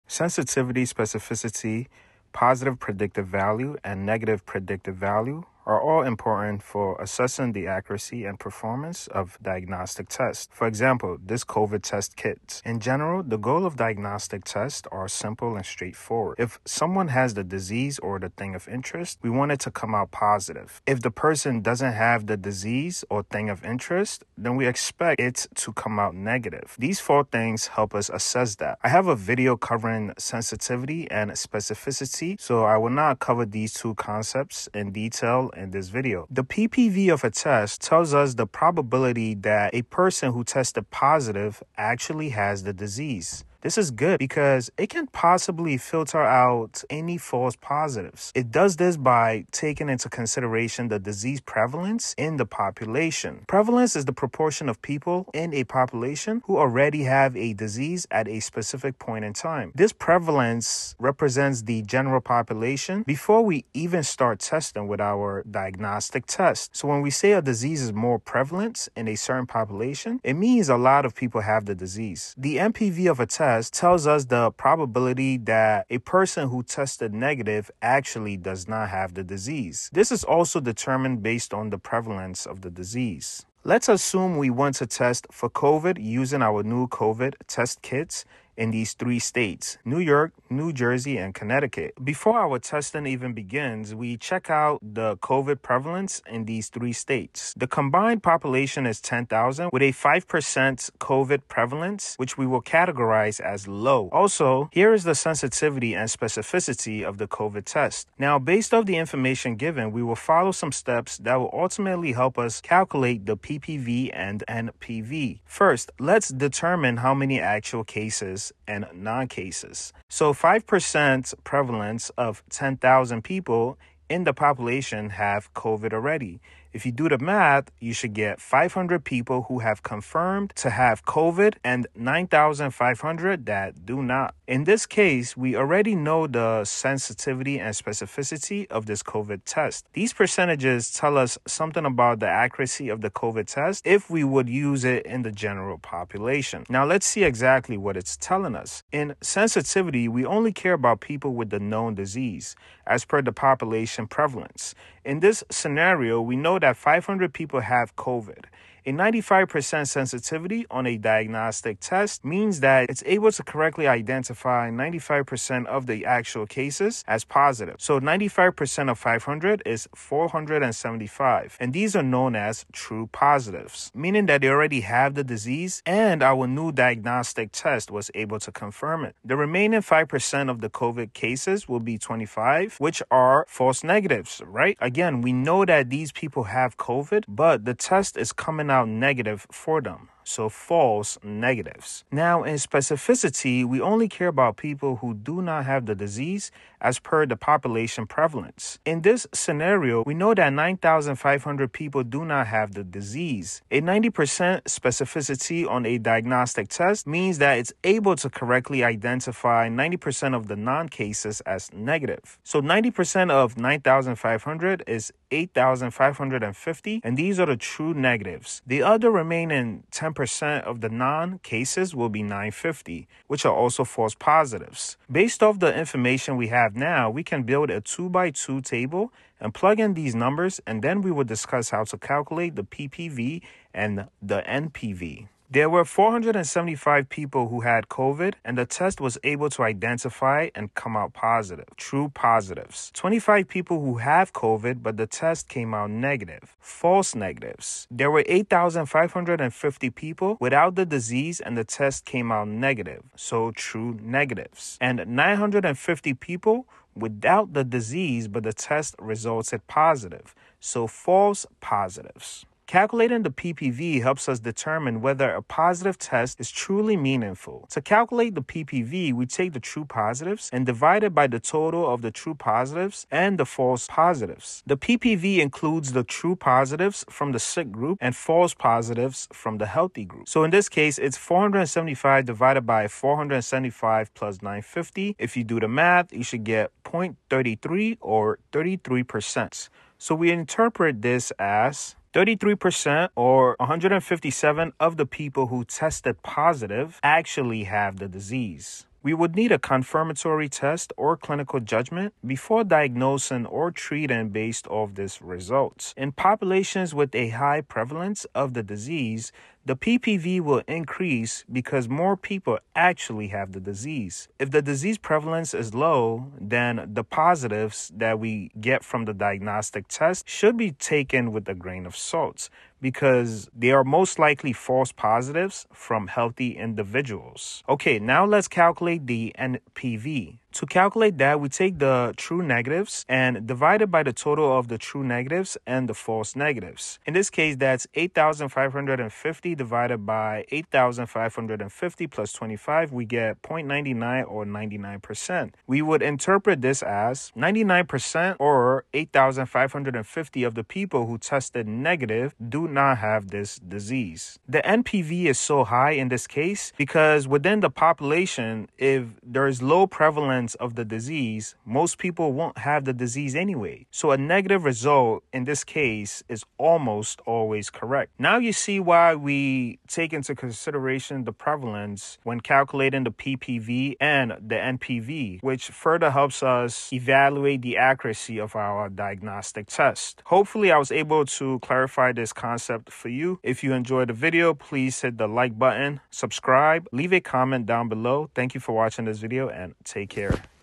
Positive Predictive Value VS Negative Predictive Value (You’re about to become an expert in this)
Confused about Positive Predictive Value (PPV) and Negative Predictive Value (NPV)? 🤔 You're not alone! In this video, I break down the key differences between these two important concepts used in statistics, medicine, and data science.

You'll learn: ✅ What PPV and NPV really mean
✅ How to calculate them
✅ Why they matter in real-world scenarios like medical testing
✅ How prevalence can affect their values

Whether you're a student, healthcare professional, or data enthusiast, this video will help you grasp these concepts with clear examples and visual explanations.